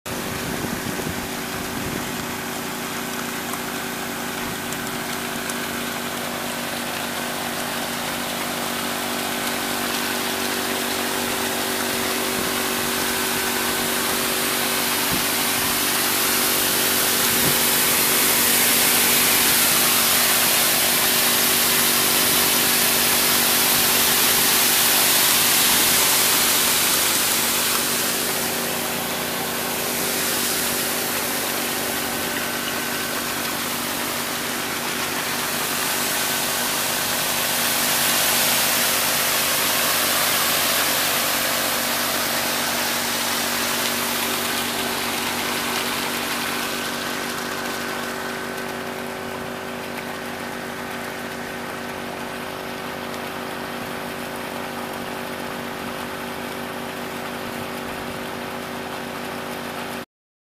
Gold- und Mineralienwaschen Sommer 2013
Ausrüstung: Keene Marlex Highbanker,
Motorpumpe: Honda 2,5 PS Keene Umbau,
Tauschsystem: Voretxmatte, Streckgitter 2x, Riffelblech original Keene.